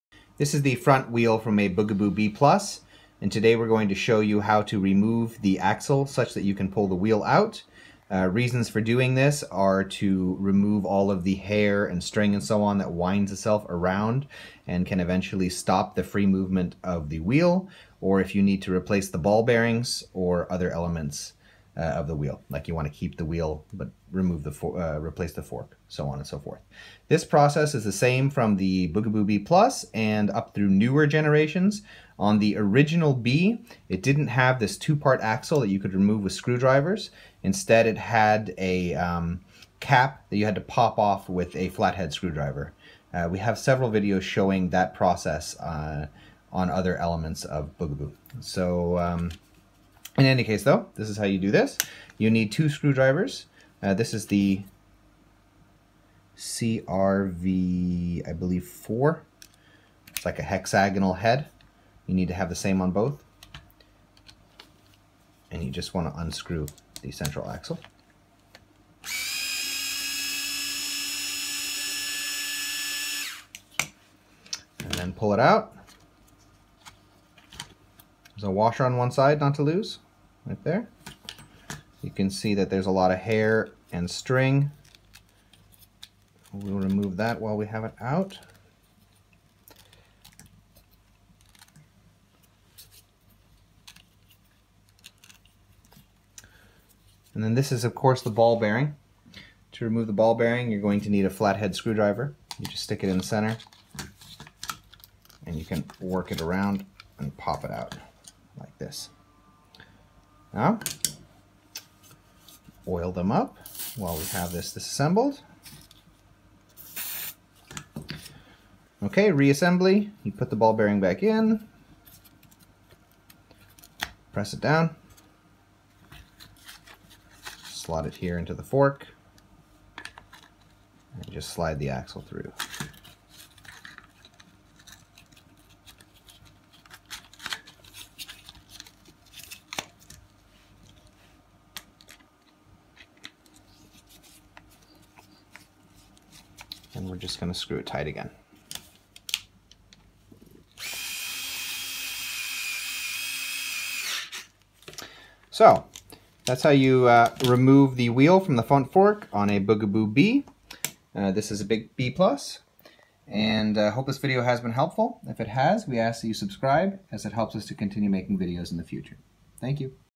Disassembling the Front Wheel on Bugaboo Bee Models
Subcribing helps preserve the longevity of our channel and ensures that we can continue to make helpful videos in the future.

In this video we demonstrate how to remove the front wheel of Bugaboo Bee models from the wheel fork, such that hair and grit can be cleaned or in order to replace ball bearings, the axle or other components of the front wheel.  We use a Bee Plus in this video, but the process applies to other generations as well (though it is slightly different with the first generation).